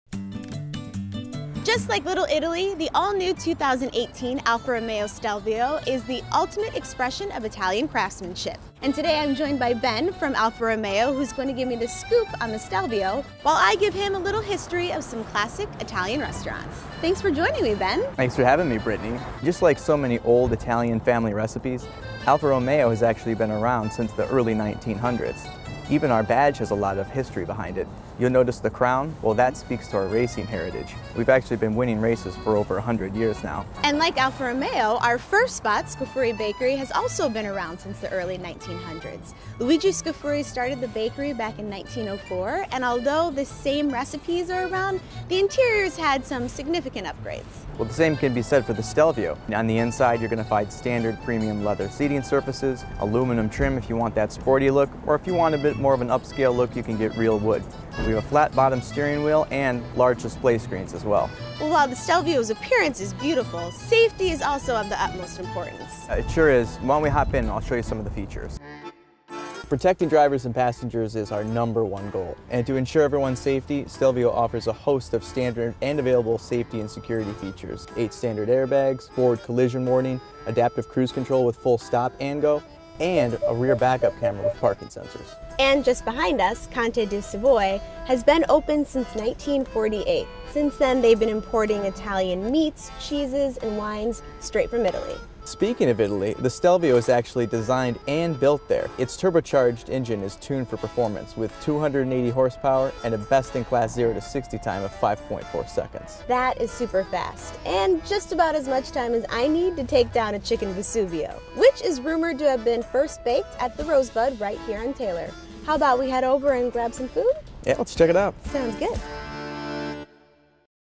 The Italian Heritage of the Alfa Romeo Stelvio
Sponsored Content: Brittney Payton takes a tour of Chicago's Historic Little Italy neighborhood in the stylish new Alfa Romeo Stelvio.